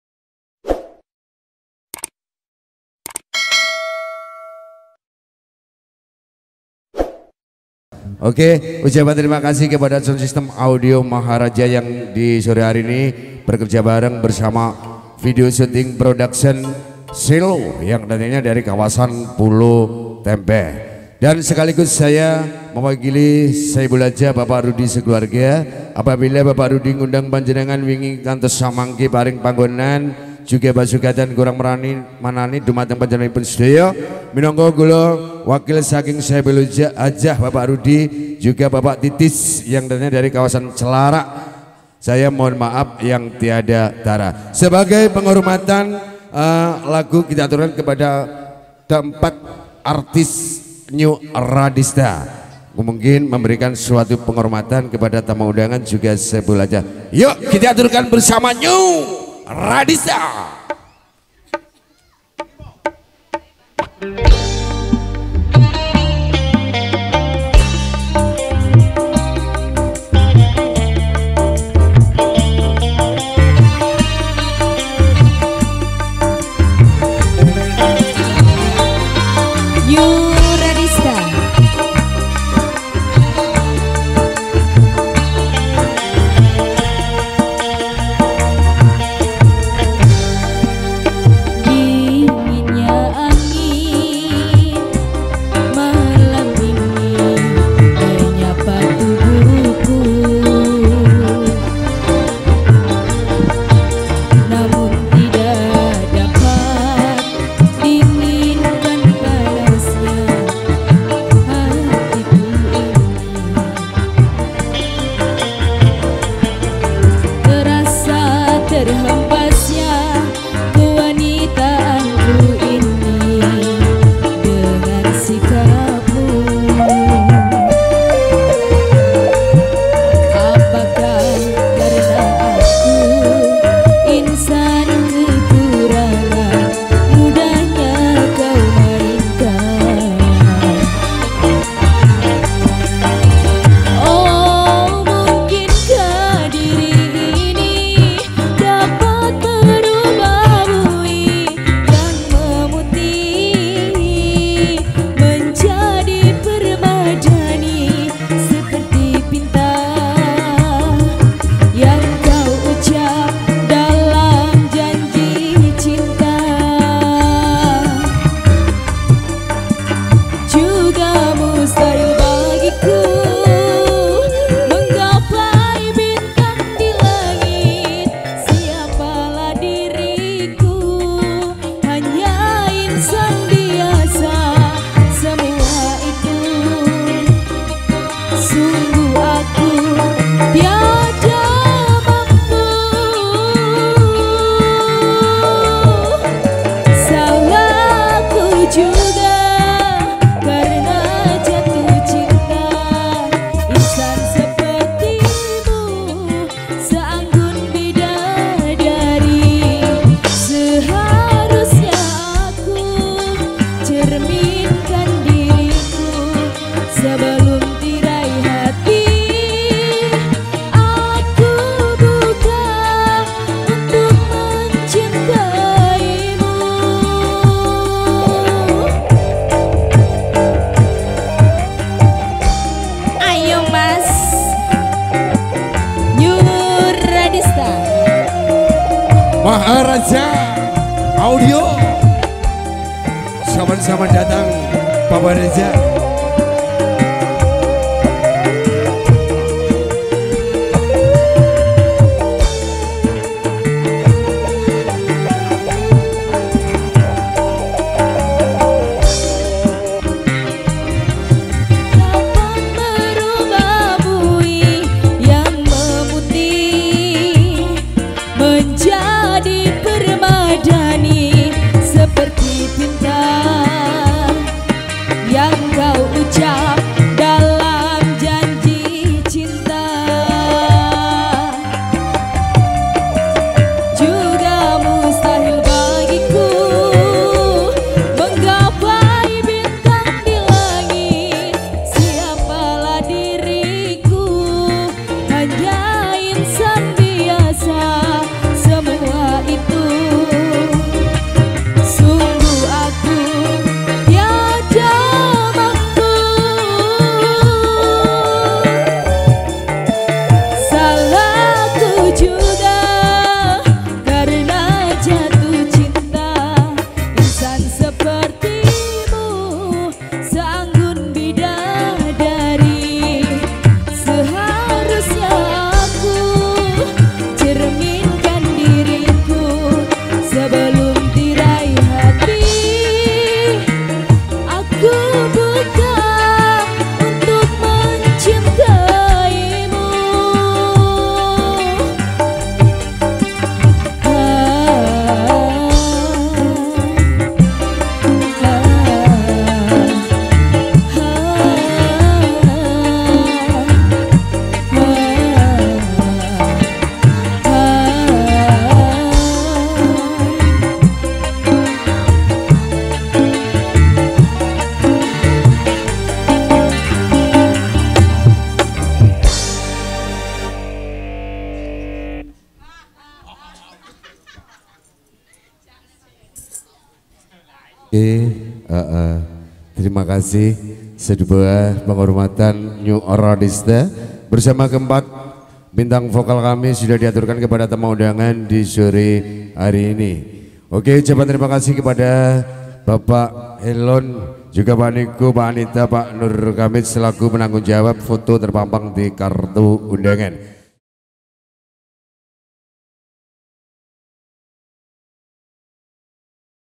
ALL Artis New RADISTA ROCK DANGDUT KOPLO Pimp. Mas Sucipto Pasirian - Lumajang
Hajat Keluarga Besar P.RUDI / TAYYIB & B. RUDI / TAYYIB
Kamis, 10 Maret 2022
Wonogriyo Kec. Tekung Kabupaten. Lumajang